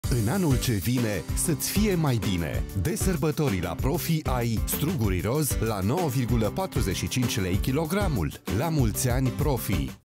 In anul ce vine, sa-ti fie mai bine! De Sarbatori, la Profi ai Stuguri roz la 9,45 lei/kg.
In anul ce vine, sa-ti fie mai bine! De Sarbatori, la Profi ai Stuguri roz la 9,45 lei/kg. Oferta supusa unor termene si conditii. La mulți ani profi!